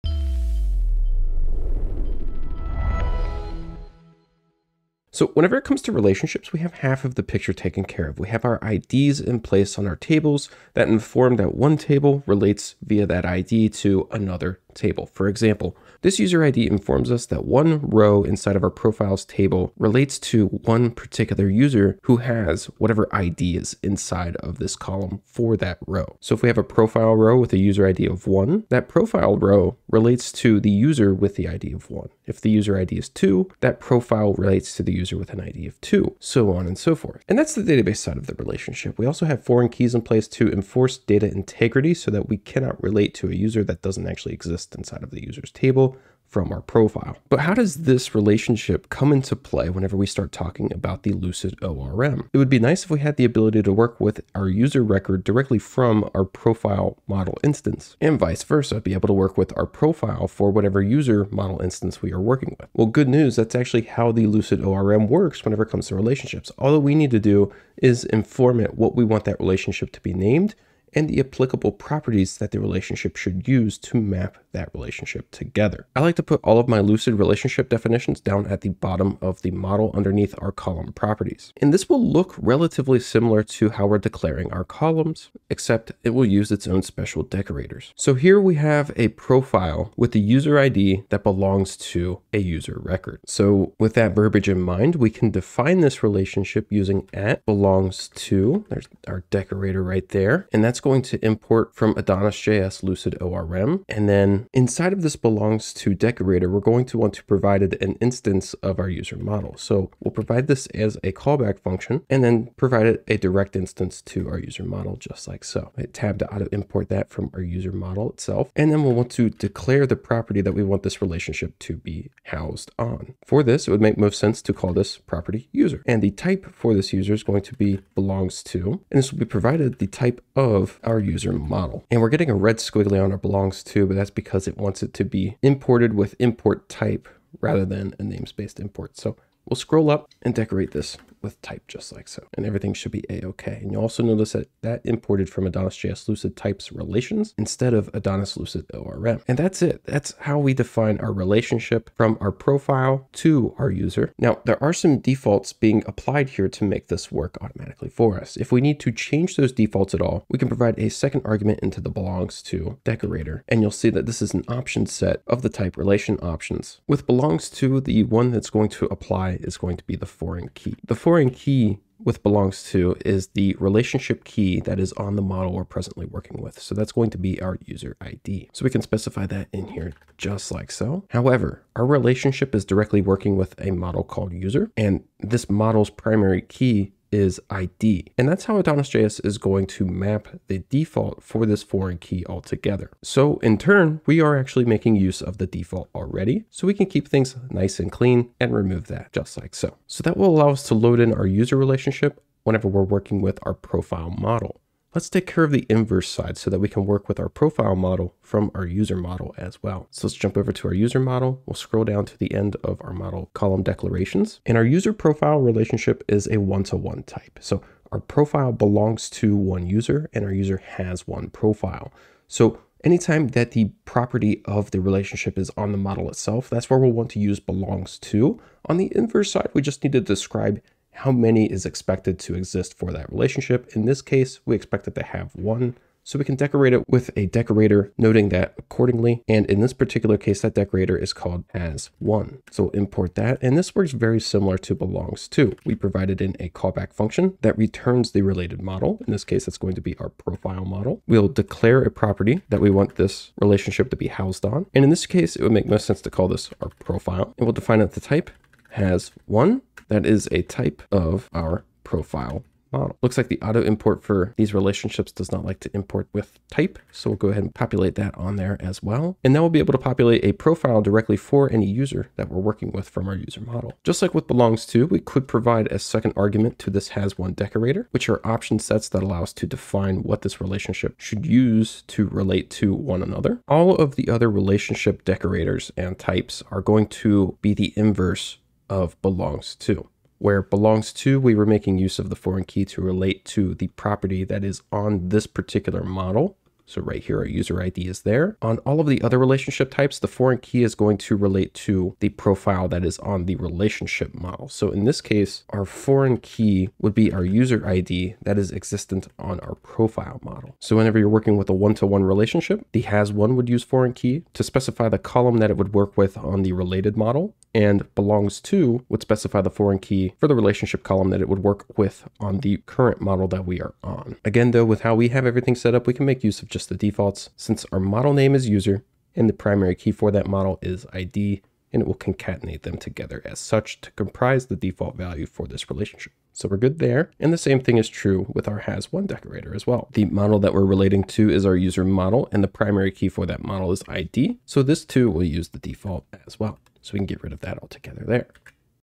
Let's Learn AdonisJS 6: Ep 5.0 - Defining One-to-One Relationships Within Lucid Models
In this lesson, we'll learn how to define one-to-one relationships within our Lucid Models. We'll learn about the belongs to and has one decorators, their options, and types that make this possible.

Socials